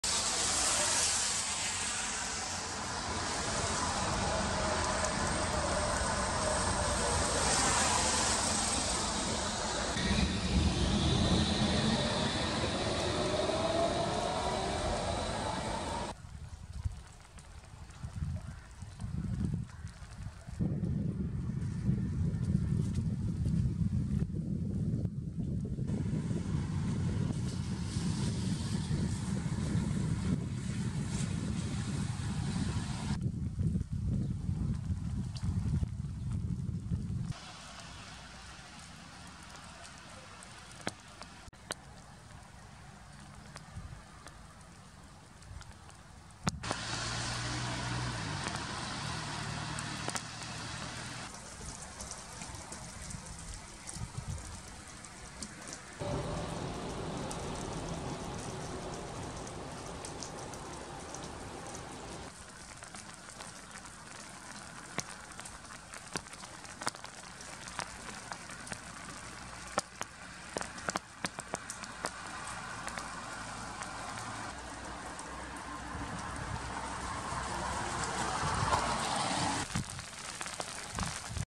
Kišni dan u Zagrebu
Najavljeno pogoršanje vremena donijelo je i u Zagrebu kišu, pad temparature i kraj ovogodišnjeg izuzetno dugog babljeg ljeta. Prema prognozama očekuje nas nestabilno razdoblje sa temperaturama primjerenim sredini jeseni.